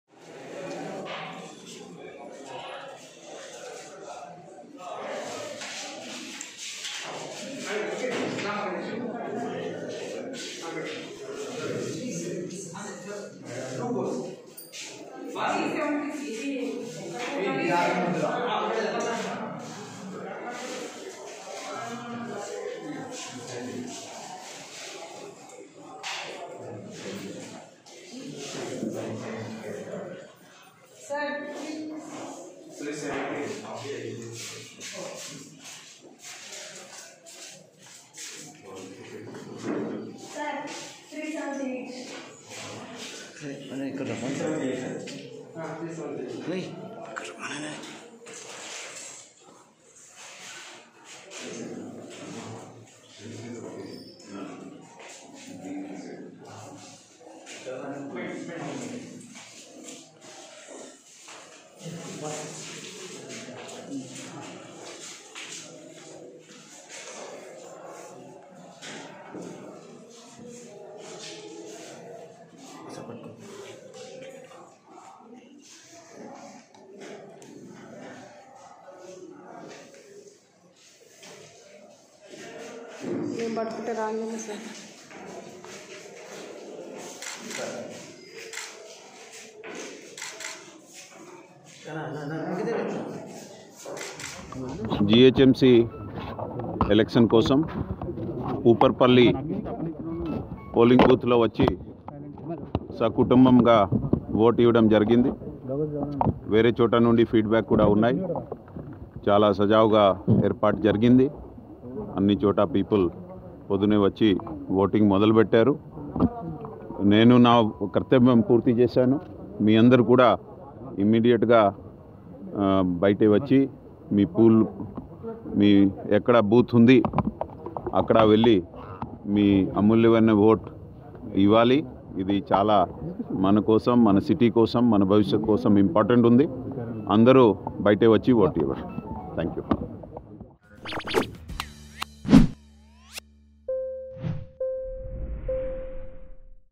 GHMC Elections 2020 Voting: ఓటు వేసిన సీఎస్ సోమేశ్ కుమార్ దంపతులు | CS Somesh kumar cast his vote
రాజేంద్ర నగర్ డివిజన్ లోని ఉప్పర్ పల్లి లో కుటుంబ సభ్యులతో కలిసి ఓటు హక్కు వినియోగించుకున్న సి.ఎస్ సోమేశ్ కుమార్.